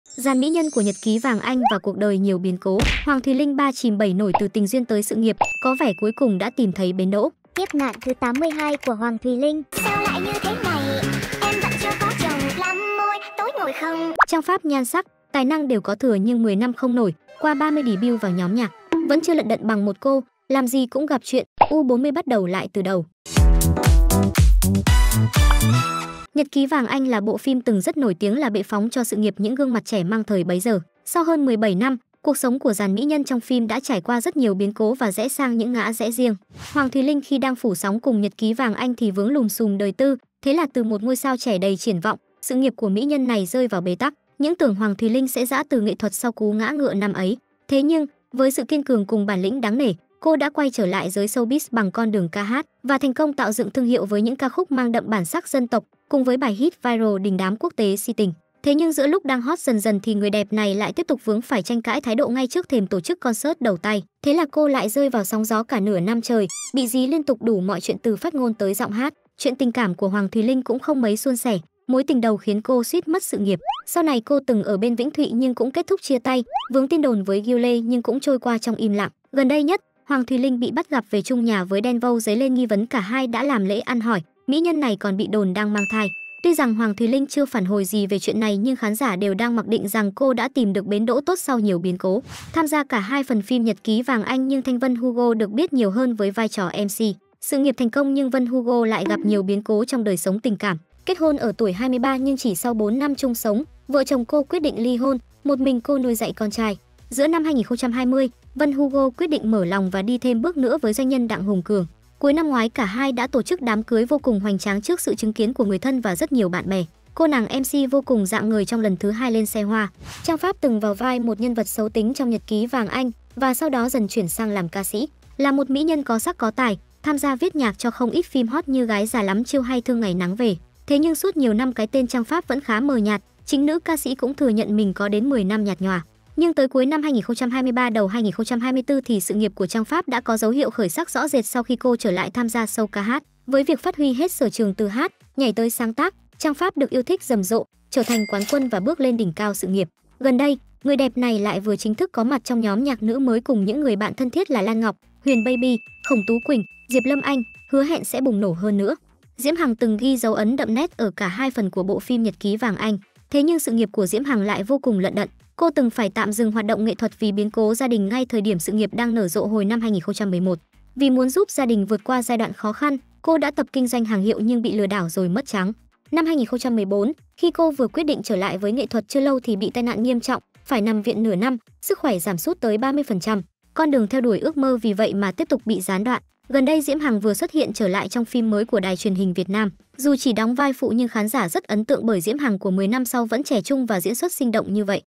Dàn mỹ nhân của Nhật Ký Vàng Anh và cuộc đời nhiều biến cố: Hoàng Thùy Linh “3 chìm 7 nổi”
Nhật Ký Vàng Anh là bộ phim từng rất nổi tiếng là bệ phóng cho sự nghiệp những gương mặt trẻ măng thời bấy giờ. Sau hơn 17 năm, cuộc sống của dàn mỹ nhân trong phim đã trải qua rất nhiều biến cố và rẽ sang những ngã rẽ riêng.